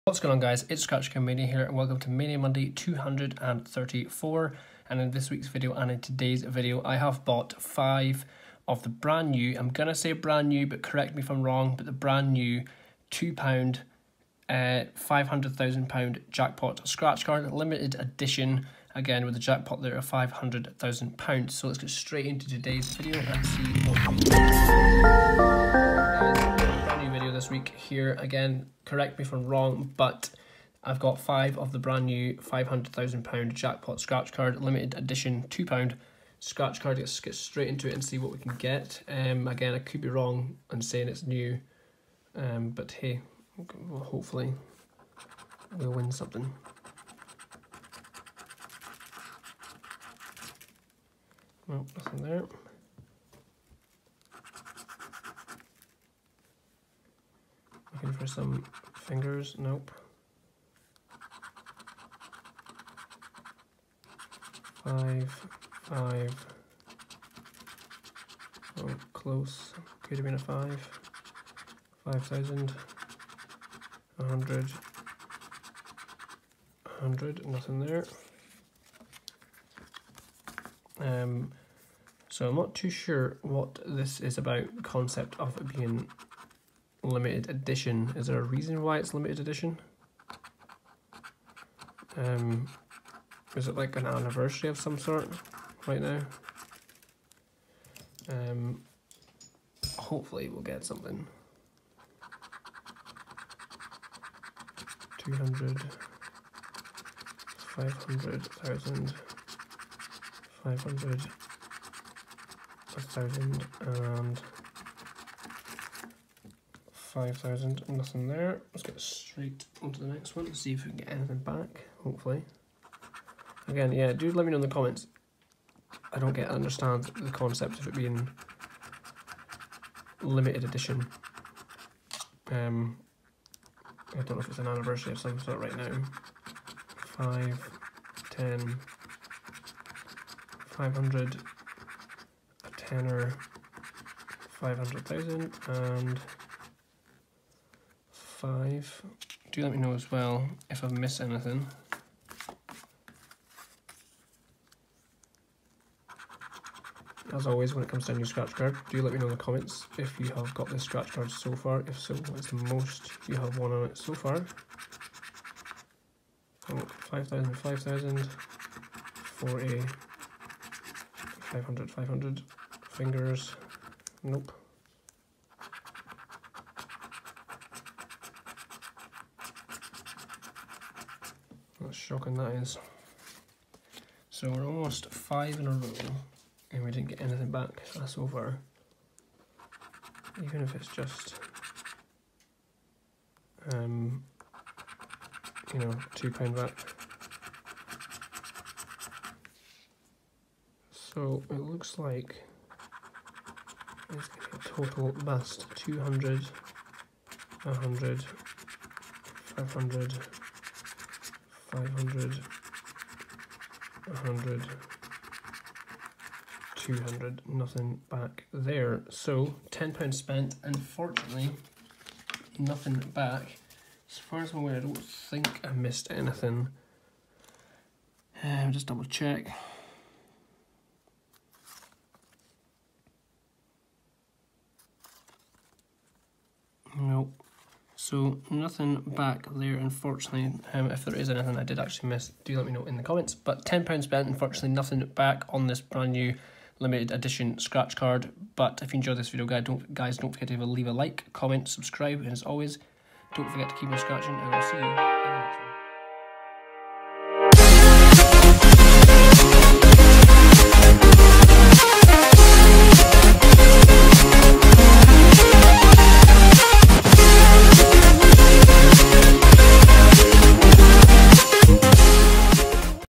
Mania Monday #234: NEW Limited Edition £2
Here with are with this weeks video. Today i bought 5 of the NEW Limited Edition £2 Card. Let’s see what we can get back!